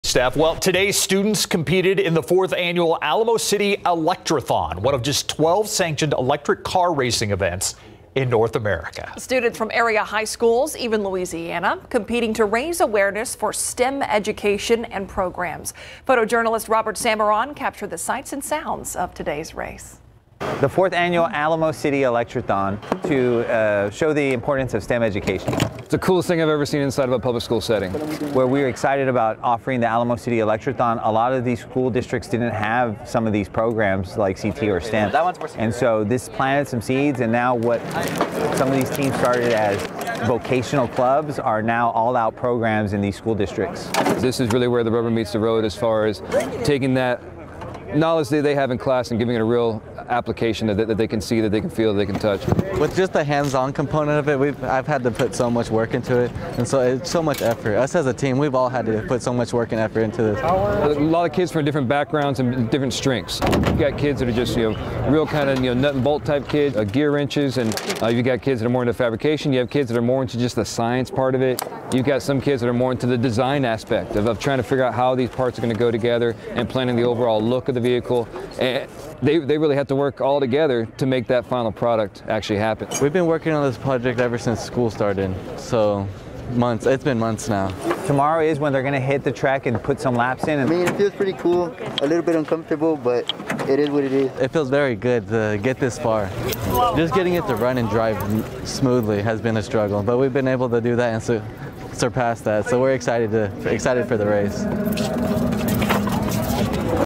Local high school students compete in 4th annual Alamo City Electrathon
Students competed in the 4th annual Alamo City Electrathon on Friday. This race is one of just 12 sanctioned electric car racing events in North America.  Students from area high schools and even Louisiana competed to raise awareness for STEM education and programs. Photojournalist Robert Samarron captured the sights and sounds of today's race.